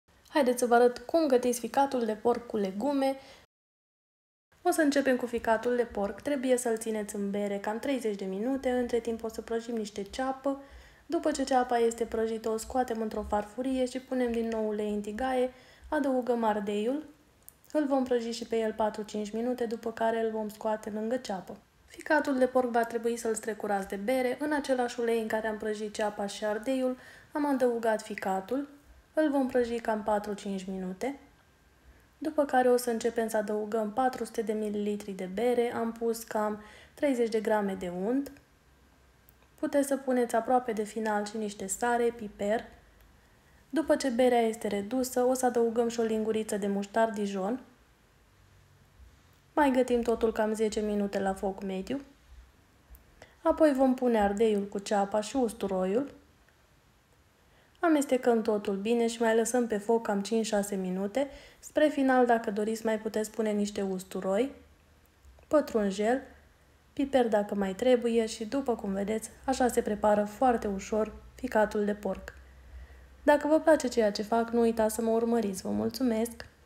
Ficat de porc cu bere si legume la tigaie, cum sa faci ficat fraged de porc.
Ficat de porc cu bere si legume, o sa iti placa aceasta reteta simpla gata in 40 de minute
🌸400 de gr ficat de porc
🌸450 ml bere
🌸2 lingurite mustar dijon
🌸1 lingurita unt
🌸2 cepe
🌸2 ardei
🌸sare piper dupa gust
🌸2-3 catei de usturoi
🌸patrunjel